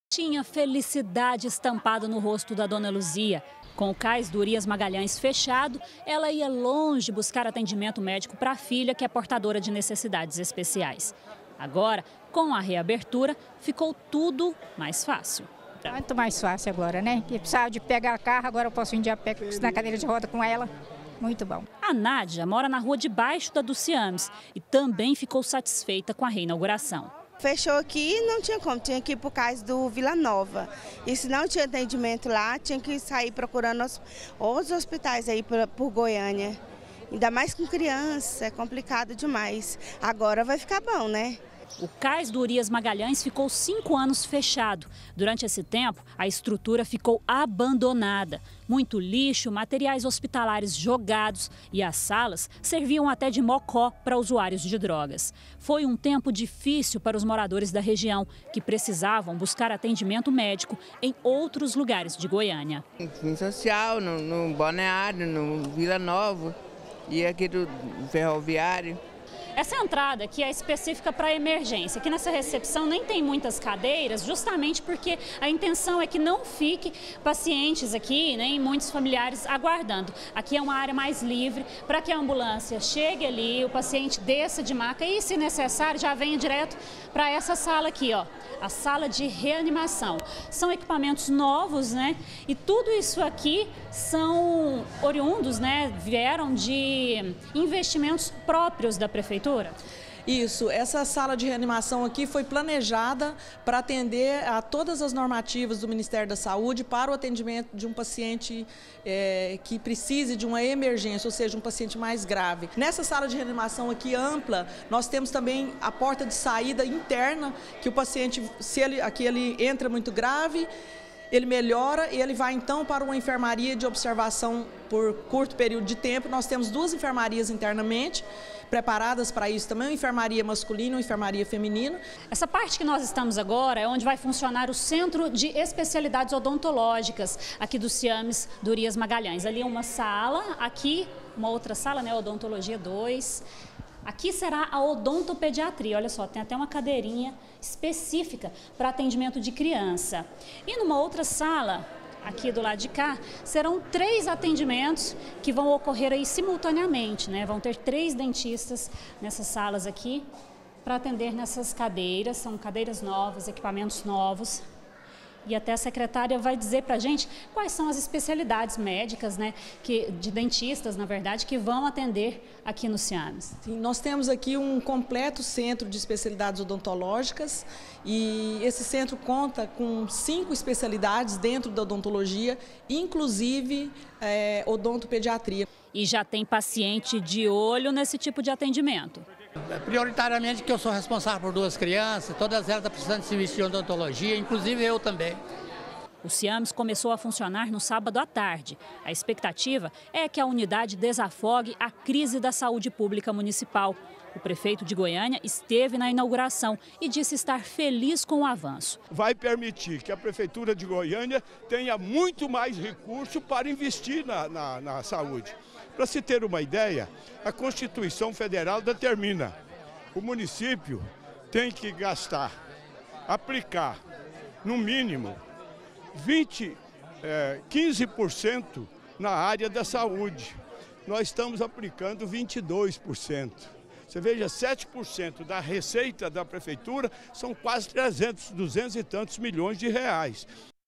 BG - CAIS do St. Urias Magalhães é reinaugurado - 17-02-2018
Moradores do Setor Urias Magalhães comemoraram a reabertura do CAIS, que ficou fechado por cinco anos. O prefeito  Iris Rezende participou do evento e garantiu que cerca de vinte e dois por cento do dinheiro do município é investido na saúde.